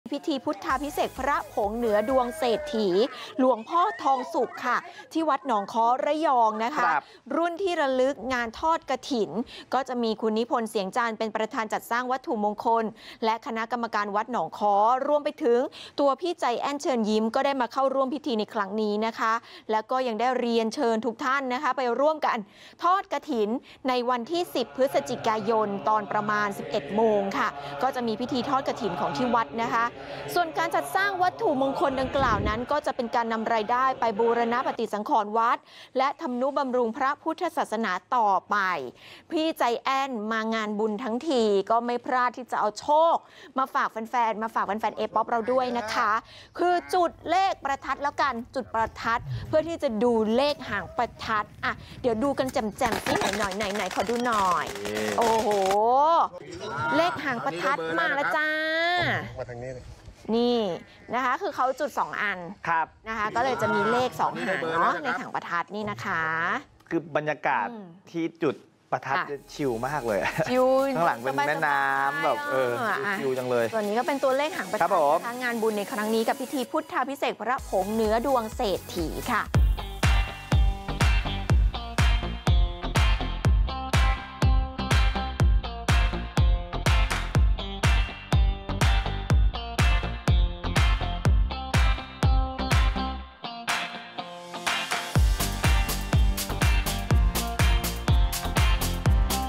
“ไจแอนท์” ร่วมงานพิธีพุทธาภิเษกพระผงเหนือดวงเศรษฐี | Apop Today
วัดหนองฆ้อ อ.บ้านค่าย จ.ระยอง มีพิธีพุทธาภิเษกพระผงเหนือดวงเศรษฐี หลวงพ่อทองสุข รุ่นที่ระลึกงานทอดกฐิน ซึ่งมี นายนิพนธ์ เสียงจันทร์ ประธานจัดสร้างวัตถุมงคล และคณะกรรมการวัดหนองฆ้อ และพี่ไจแอนท์ เชิญยิ้ม ได้เข้าร่วมพิธี เรียนเชิญประชาชนมาร่วมทอดกฐิน วันที่ 10 พ.ย. เวลา 11.00 น. จะมีพิธีทอดกฐิน ทั้งนี้การจัดสร้างวัตถุมงคลดังกล่าว จะนำรายได้บูรณะปฏิสังขรณ์วัด และทำนุบำรุงพระพุทธศาสนาสืบไป